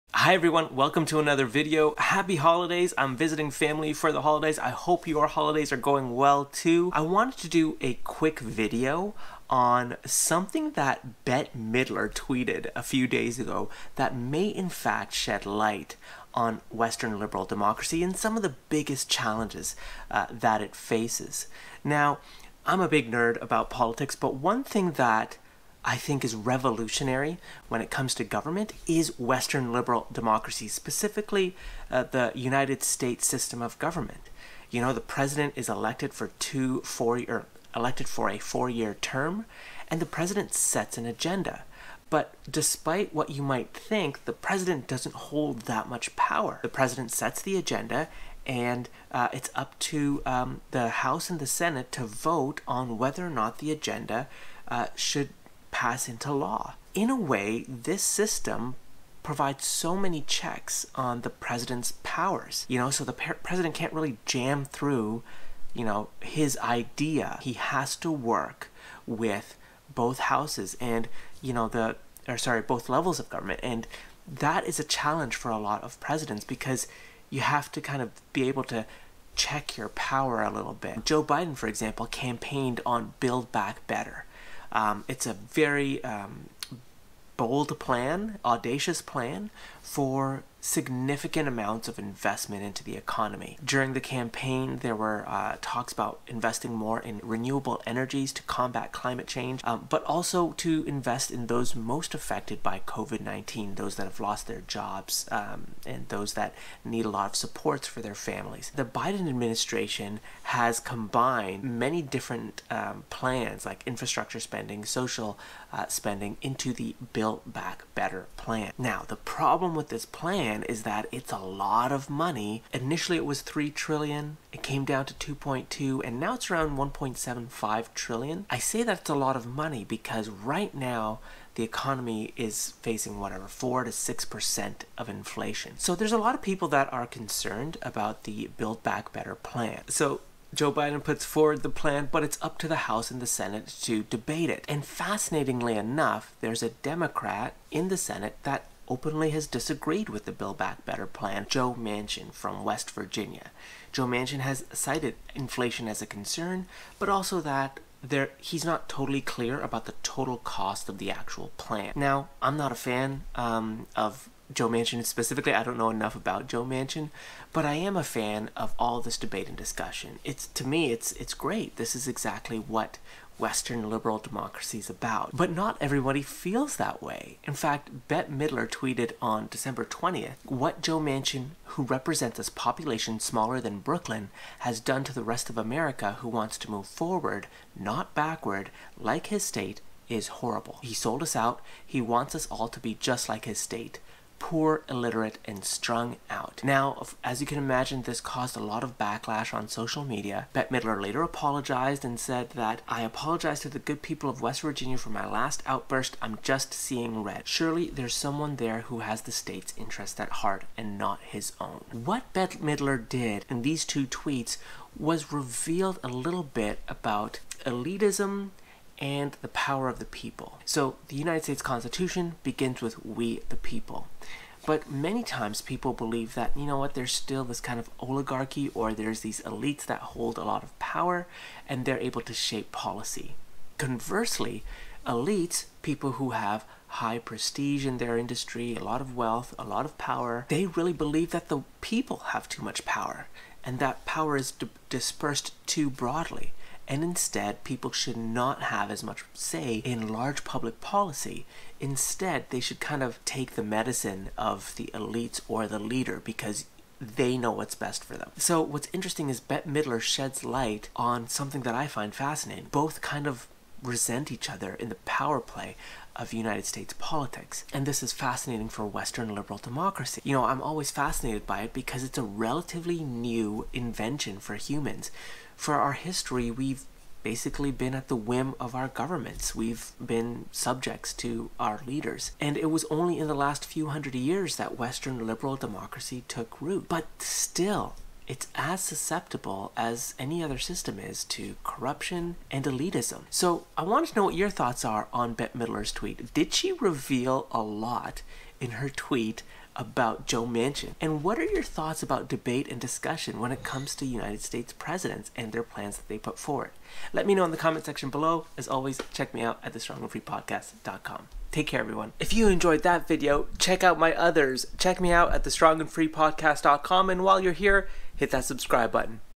Did Bette Midler reveal more about our DEMOCRACY than she thought?
Did Bette Midler reveal more about our democracy in two simple Tweets than she thought?!

I've done series on all types of content. Find it wherever you stream music!